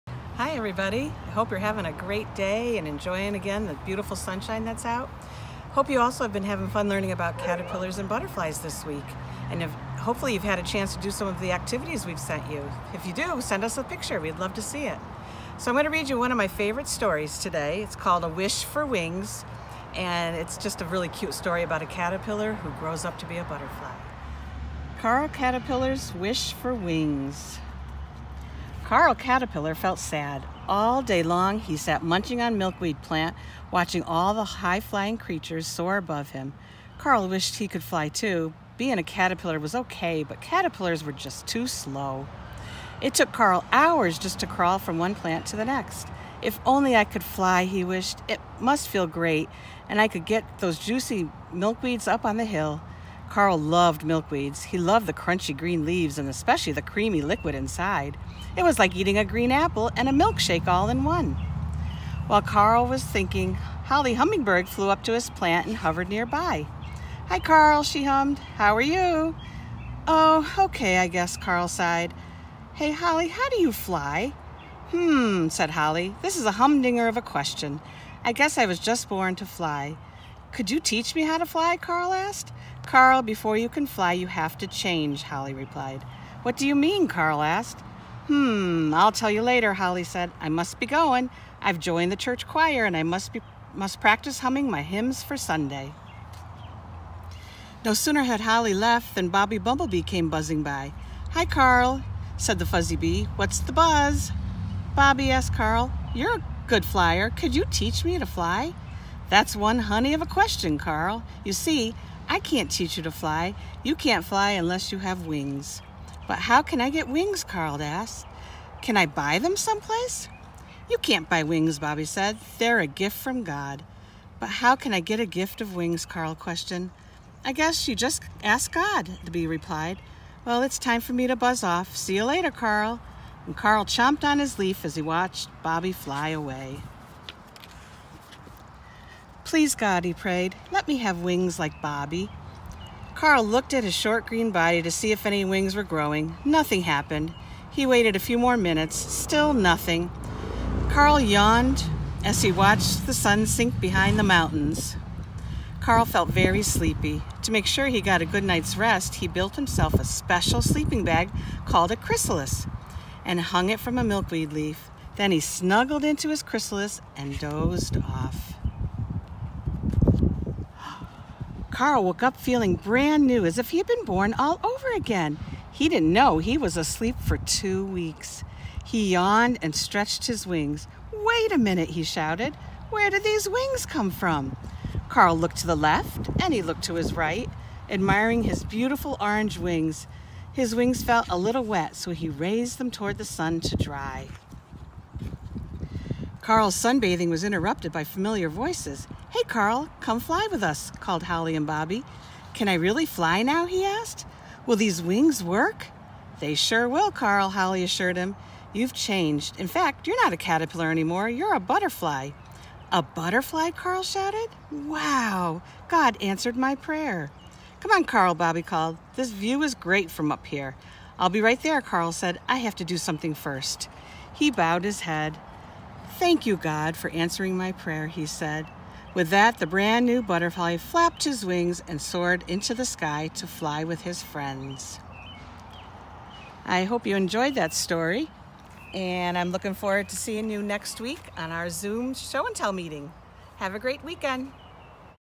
Story Time with Mrs. Hammer
Listen to this cute story about caterpillars and butterflies!